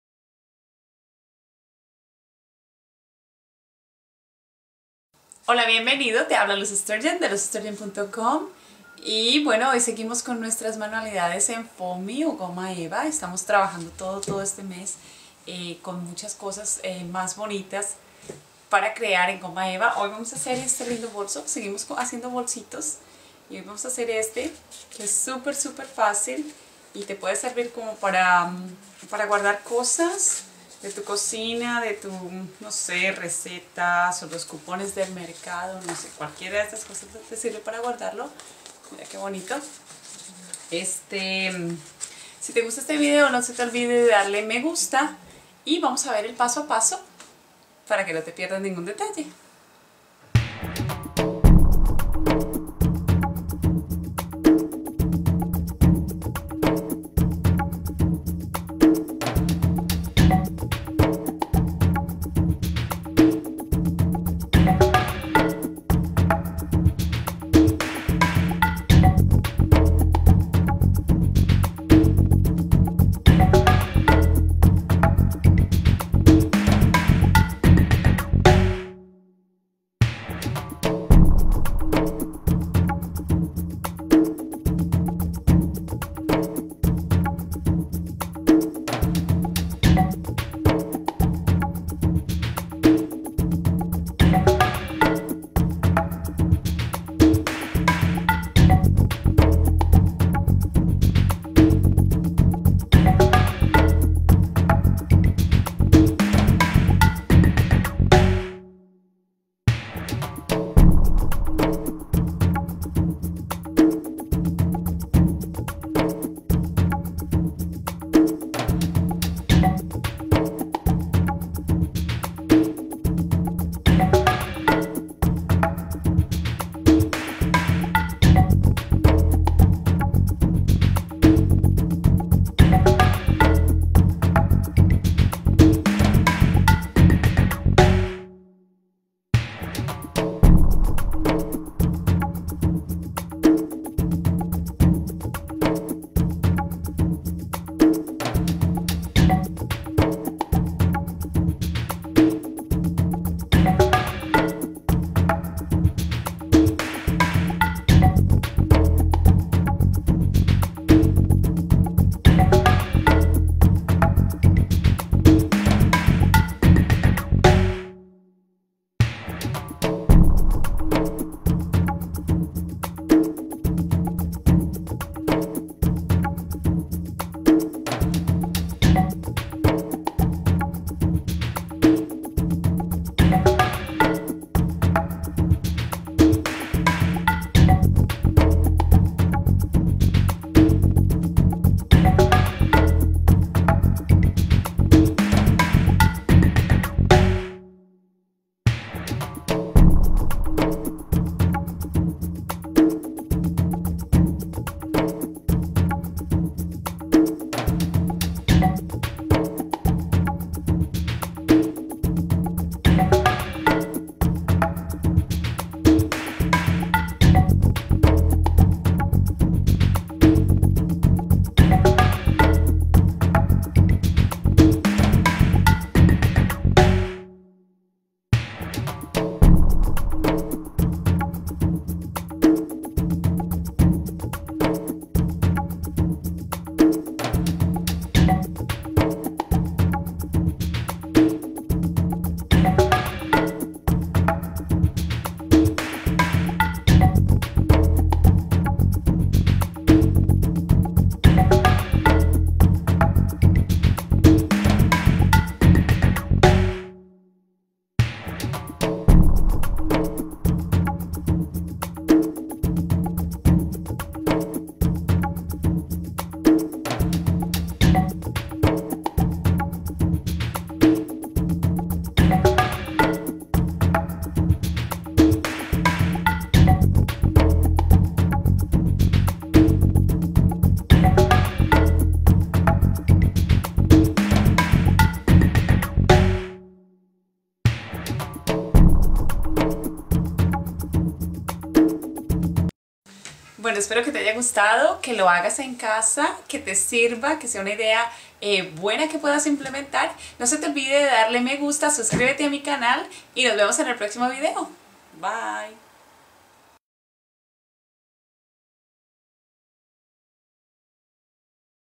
How to Craft a Purse from Foam Sheets #3 (Spanish)
Luz Sturgeon shows how to craft a purse from foam sheets.